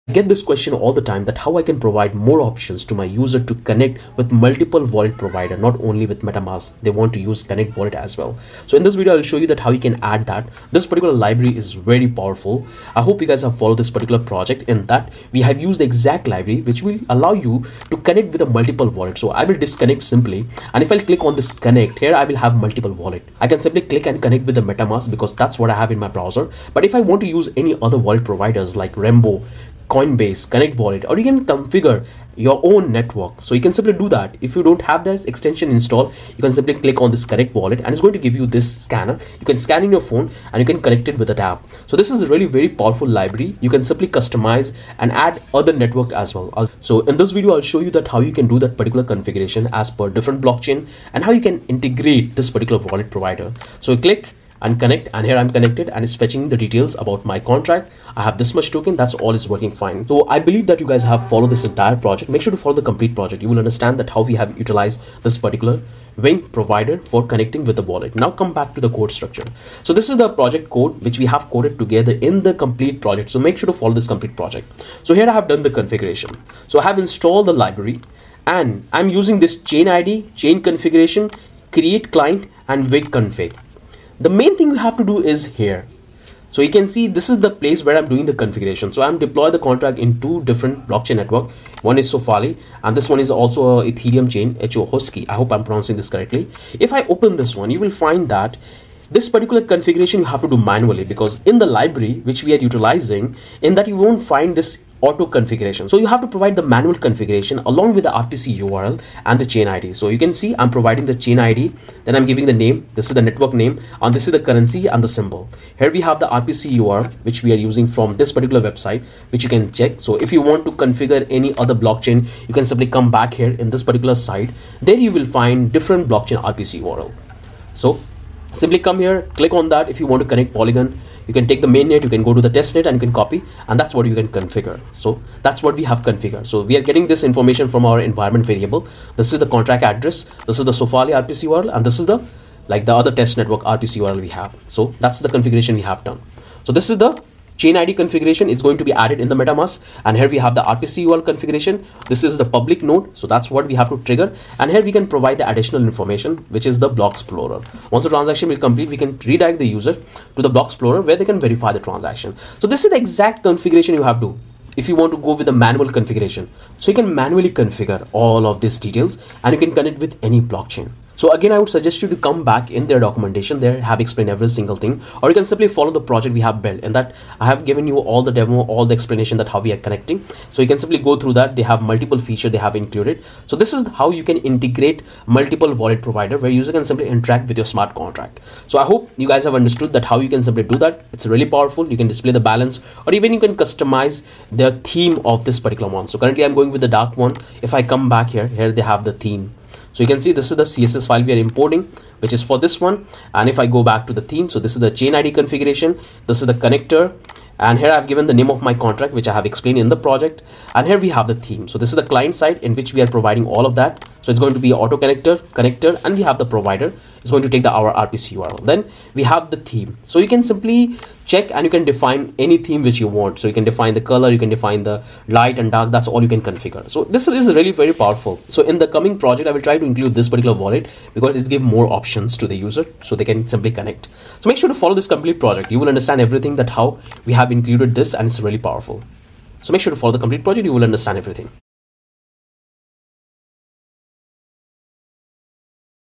Web3 Dapp Multi Crypto Wallet Provider | Wagmi Reactivity for Ethereum Apps Project Setup
Web3 Dapp Multi Crypto Wallet

"Welcome to our comprehensive guide on setting up a Web3 Dapp Multi Crypto Wallet Provider using Wagmi Reactivity for Ethereum applications! In this tutorial, we'll walk you through the essential steps to integrate multiple crypto wallets into your decentralized app, ensuring a seamless and secure user experience.

🔹 What You'll Learn:

Introduction to Web3 and Dapps
Setting up a multi crypto wallet provider
Integrating Wagmi Reactivity for enhanced Ethereum app functionality
Best practices for secure wallet management
🔹 Why Choose Wagmi Reactivity?
Wagmi provides a powerful and reactive framework that simplifies the integration of Ethereum wallets into your Dapp. Its flexibility and ease of use make it an excellent choice for both beginners and experienced developers.

🔹 Get Started:

Prerequisites: Basic knowledge of JavaScript and Ethereum
Setting up your development environment
Step-by-step project setup and configuration
🔹 Resources:

Wagmi Documentation
Ethereum.org
Web3.js Guide
💬 Join the Discussion:

Workout Video: